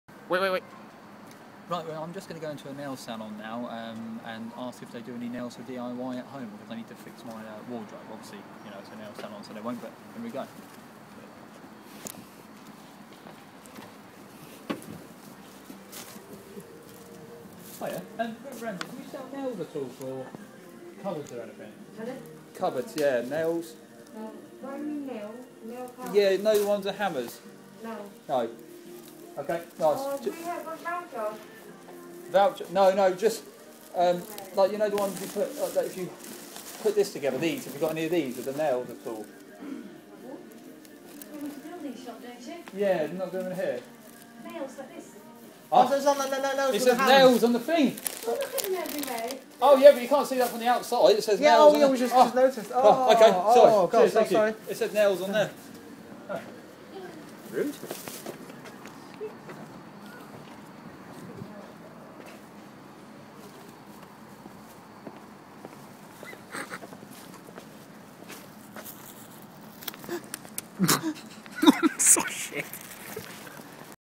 Funny prank funniest prank
Asking for DIY nails in a nail salon
I decided to go into a nail salon and ask if they have any DIY nails that you use around the houseThey were confused and told me no.Then a lady said you need  do it yourself shop they don't have any here and told us to get out was very funny and I hope you enjoy the video

Funniest prank
top prank
top pranks